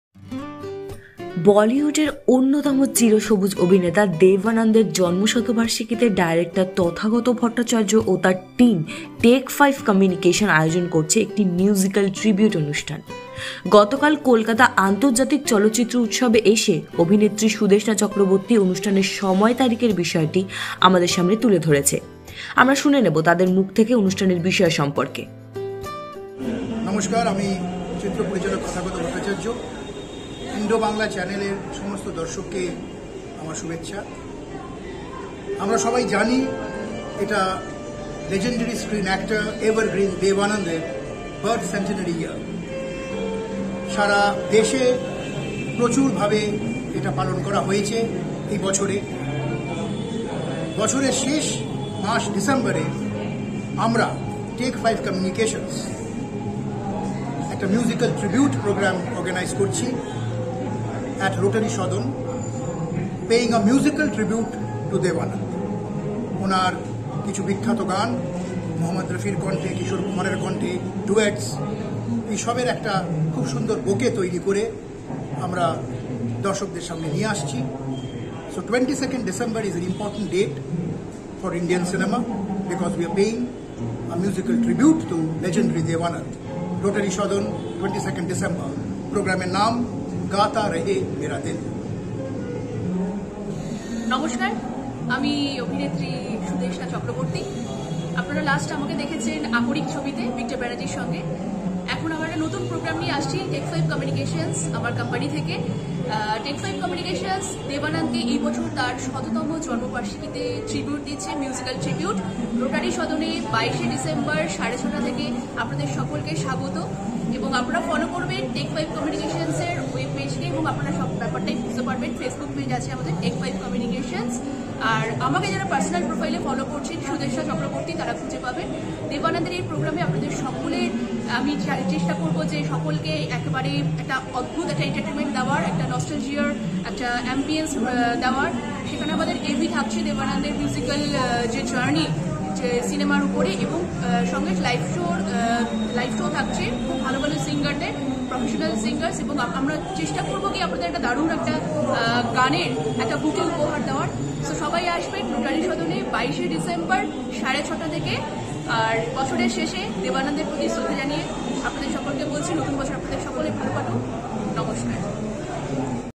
Dev Anand: সংগীত স্মরণে দেব আনন্দ, দেখুন | Bangla News
Dev Anand: সংগীত স্মরণে দেব আনন্দ, দেখুন। বলিউডের অন্যতম চির সবুজ অভিনেতা দেব আনন্দের জন্মশতবার্ষিকীতে ডাইরেক্টর তথাগত ভট্টাচার্য ও তার টিম টেক ফাইভ কমিউনিকেশন আয়োজন করছে একটি মিউজিক্যাল ত্রিবিউট অনুষ্ঠান। গতকাল কলকাতা আন্তর্জাতিক চলচ্চিত্র উৎসবে এসে অভিনেত্রী সুদেষ্ণা চক্রবর্তী অনুষ্ঠানের সময় তারিখের বিষয়টি আমাদের সামনে তুলে ধরেছে। আমরা শুনে নেবো তাদের মুখ থেকে অনুষ্ঠানের বিষয় সম্পর্কে  | Bangla News